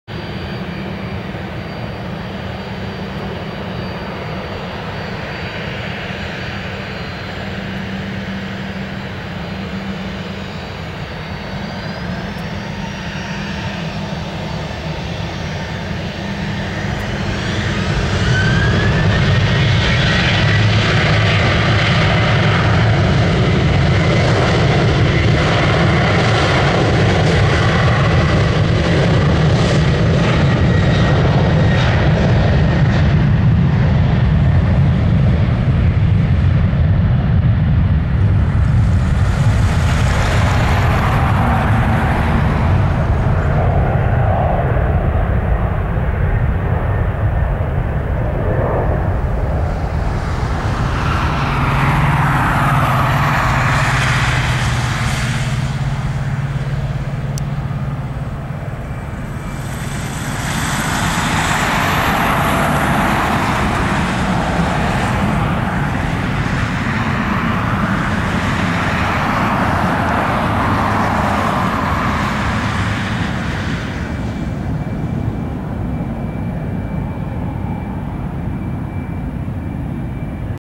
Alaska Airlines (Hawaiian Lei Tail) N508AS 737-800 Takeoff Portland Airport (PDX)
Flight: ASA809 PDX-OGG